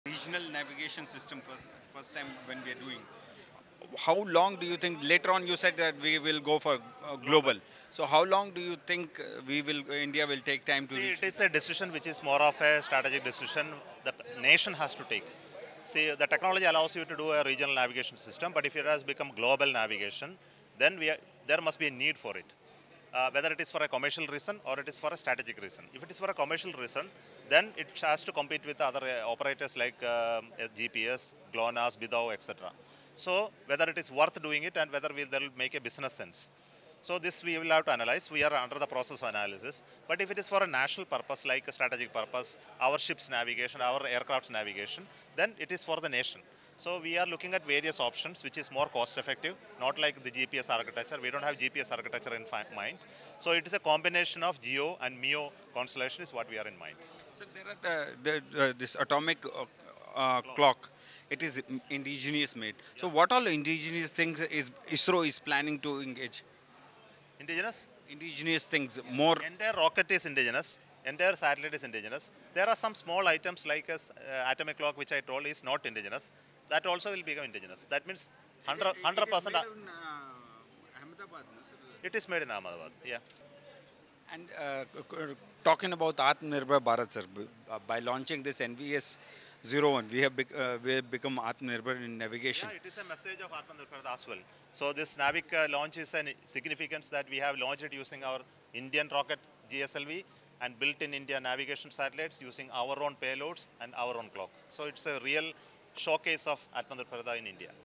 GSLV-F12 and NVS-01 | ISRO Chairman S. Somnath
By launching GSLV-F12 and NVS-01, India will become Atma Nirbhar in the field of navigation (PNT). This is a regional navigation satellite system, but in the future, we can think of making a global navigation satellite," said ISRO Chairman S. Somnath, speaking exclusively with Doordarshan. He further told me that developing a global navigation system depends on strategy and commercial purposes.